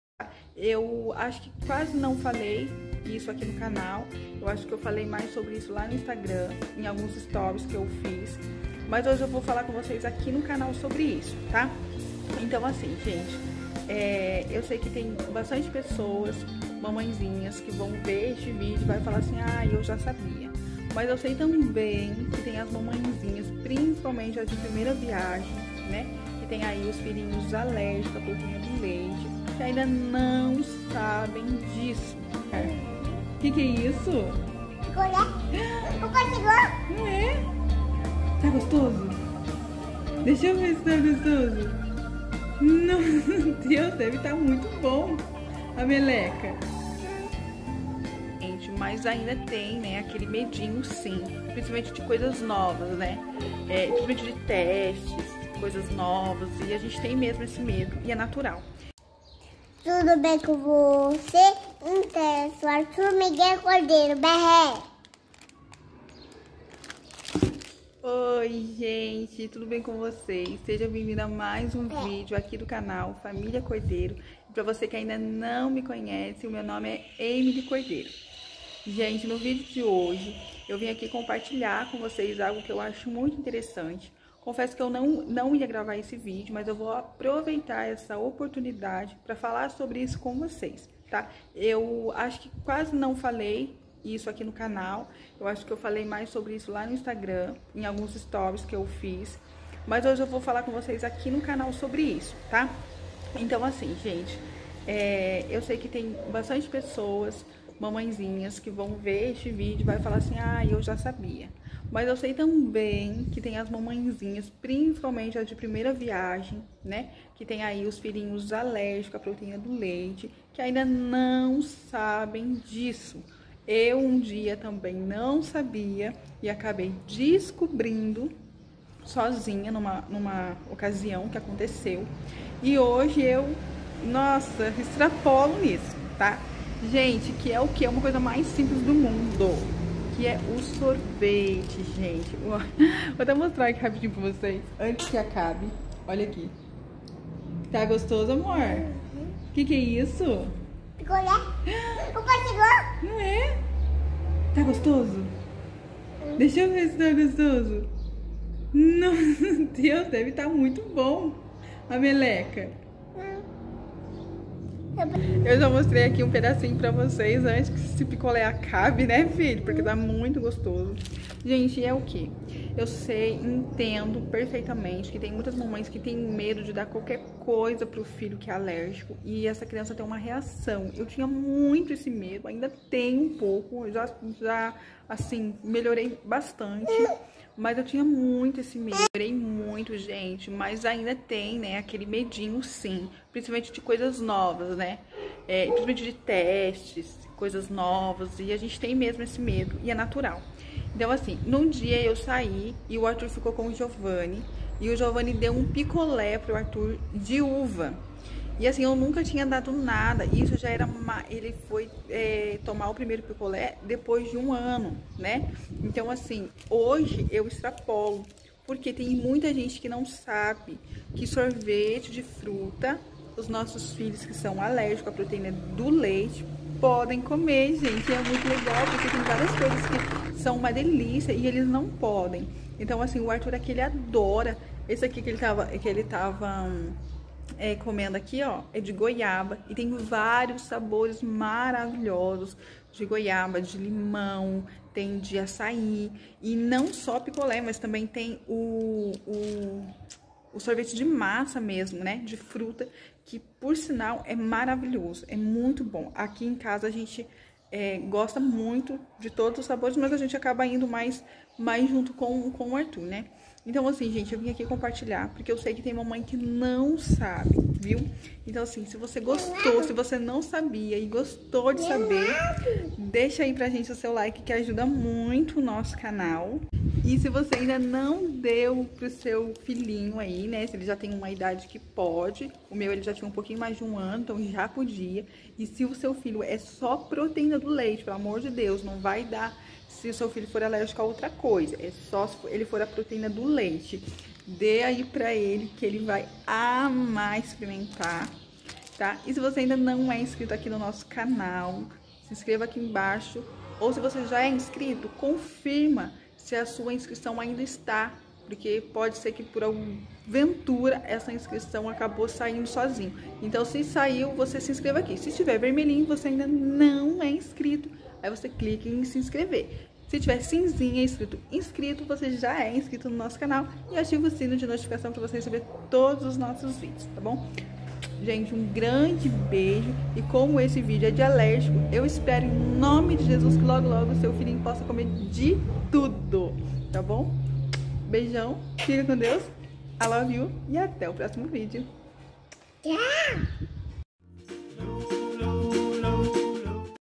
SORVETE PARA ALÉRGICO A LEITE *FAMÍLIA CORDEIROH 🐑
Oiii Gente tudo bem...
No vídeo de hoje vamos falar de um assunto muito importante, algumas mamães de APLV ainda não sabem...principalmente as de primeira viagem!
Que é sobre o sorvete para alérgicos a proteína do leite.
Espero que esse vídeo tenha ajudado ...
E quem ainda não é inscrito no canal?
Tem que se inscrever aqui embaixo ( palavras do Arthur kkk)